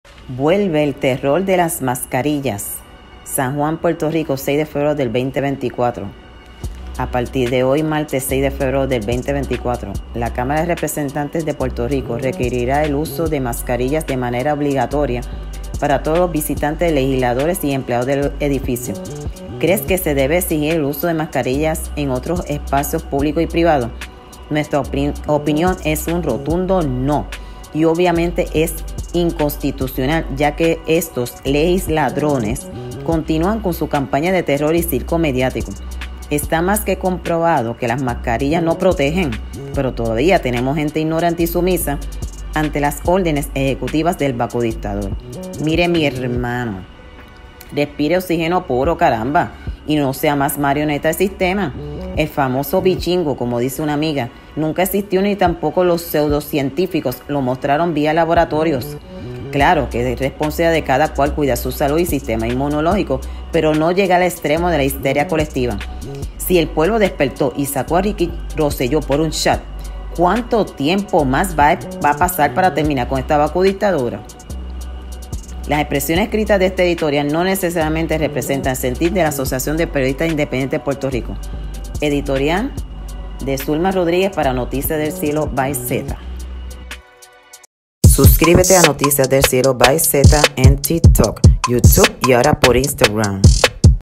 EDITORIAL: VUELVE EL TERROR DE LAS MASCARILLAS (6/FEBRERO/2024)
Y VUELVE EL CIRCO MEDIÁTICO DE LAS MASCARILLAS
San Juan PR 6 de febrero de 2024
A partir de hoy martes 6 de febrero de 2024, la Cámara de Representantes de Puerto Rico requerirá el uso de mascarillas de manera obligatoria para todos los visitantes, legisladores y empleados del edificio.
¿Crees que se debe exigir el uso de mascarillas en otros espacios públicos y privados? Nuestra opinión es un rotundo NO y obviamente es inconstitucional ya que estos legisladrones continúan con su campaña de terror y circo mediático . Está más que comprobado que las mascarillas no protegen, pero todavía tenemos gente ignorante y sumisa ante las “órdenes ejecutivas” del vacu dictador; mire mi herrrmano respire oxígeno puro, caramba y no sea más marioneta del sistema!!! El famoso “bichingo” (como dice una amiga) nunca existió ni tampoco los seudo científicos lo mostraron vía laboratorios. Claro que es responsabilidad de cada cual cuidar su salud y sistema inmunológico pero no llegar al extremo de la histeria colectiva. Si el pueblo despertó, y sacó a Ricky Rosello por un chat, cuánto tiempo más va a pasar para terminar con esta vacu dictadura???
Las expresiones escritas de este editorial no necesariamente representan el sentir de la Asociación de Periodistas Independientes de Puerto Rico.